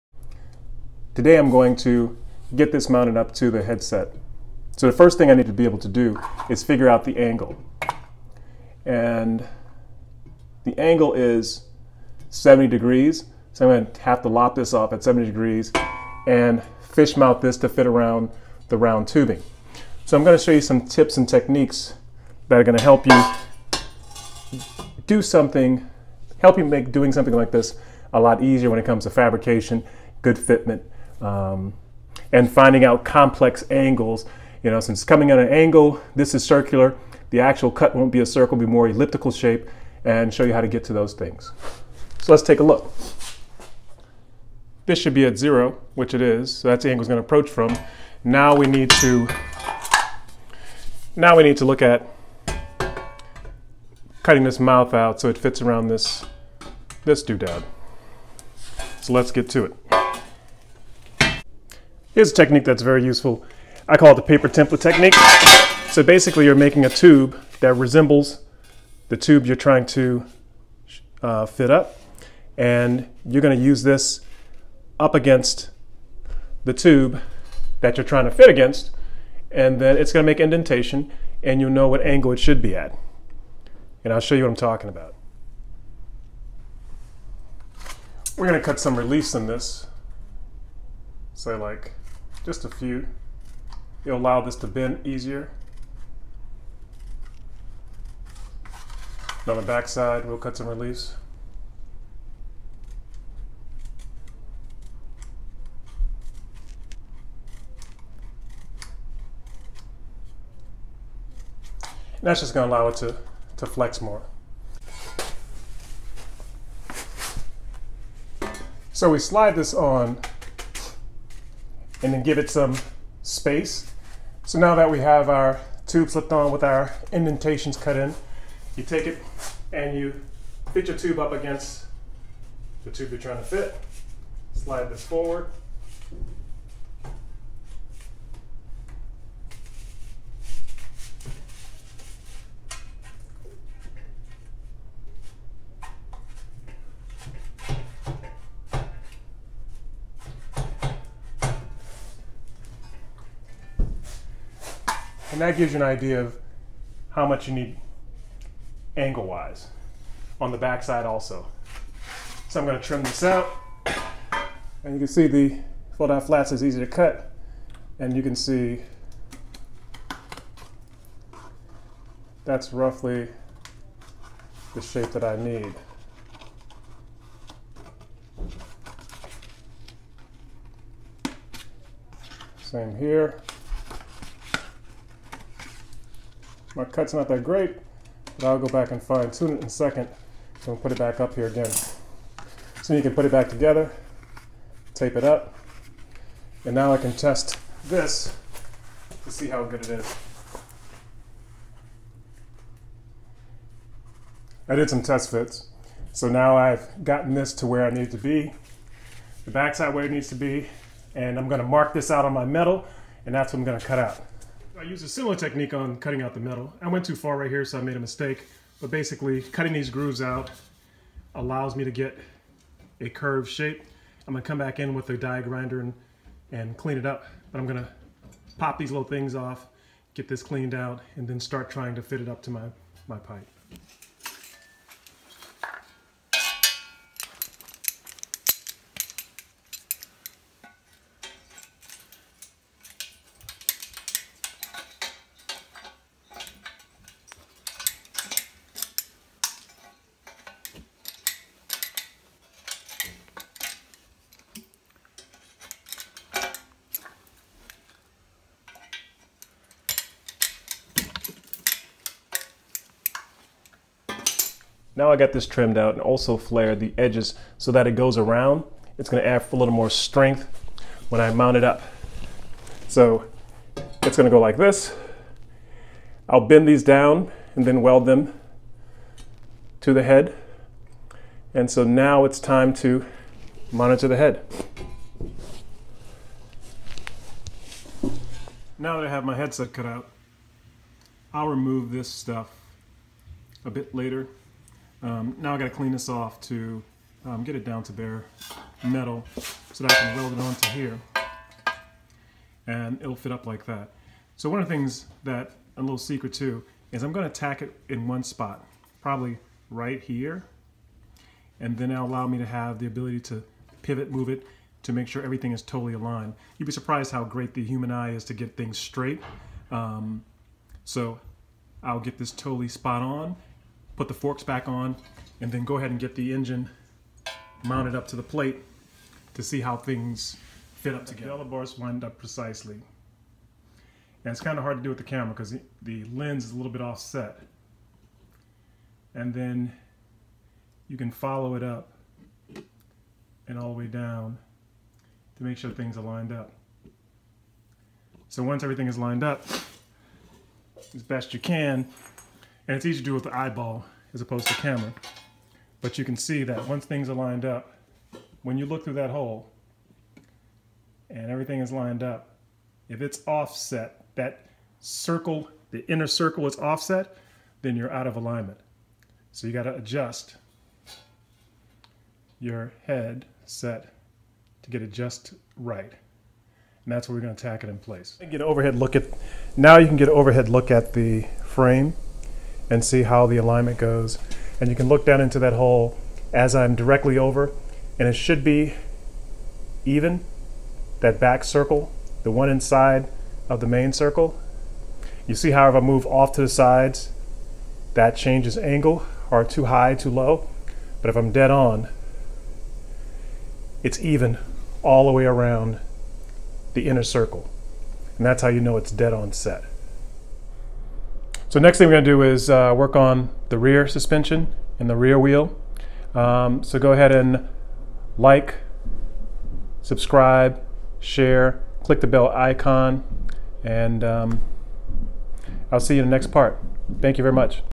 Part 3 -- Predator 212cc Powered Bike Project
Part 3: Building the frame for the mountain bike with a Predator 212cc engine as the power plant. This is part three of the project. I’m showing how to build something like this with simple tools, and problem solving.

Here are some parts that may help if you want to try this project yourself:
Predator Engine
Torque Converter
GoKart Clutch
Mini Bike Sproket